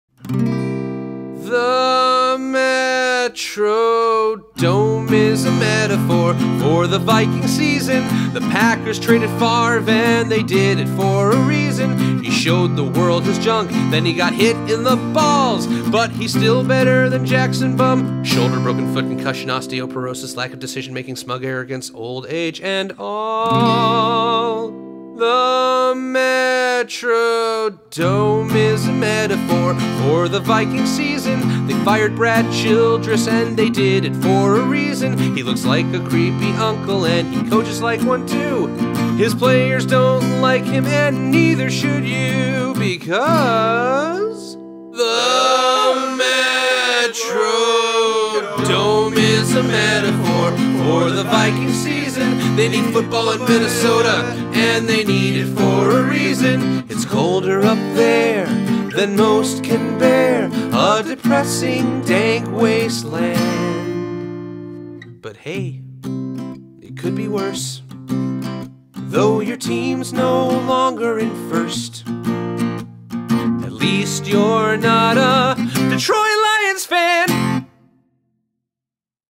The Metrodome
For those who enjoy watching Favre get hit in the nuts.
"They need football in Minnesota, and they need it for a reason."